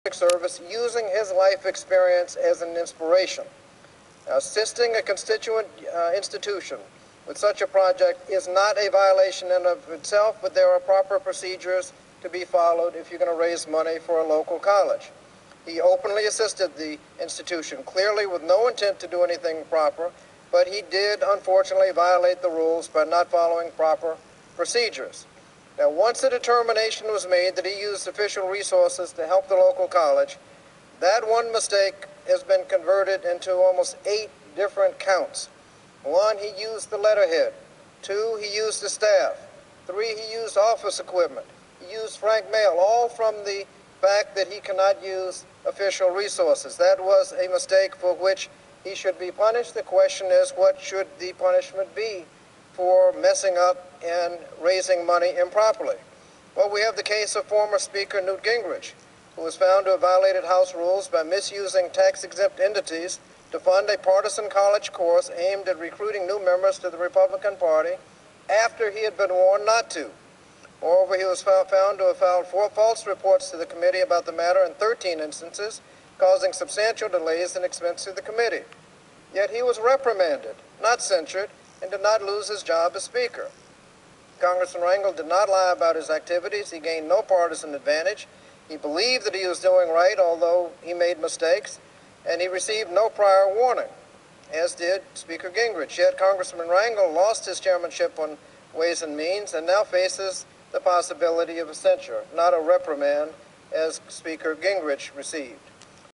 12.02.10 - Rep. Bobby Scott: Newt Reprimanded. Rangel Censured.
12.02.10 - Rep. Bobby Scott Defends Rep. Charlie Rangel Uses the Newt Gingrich example...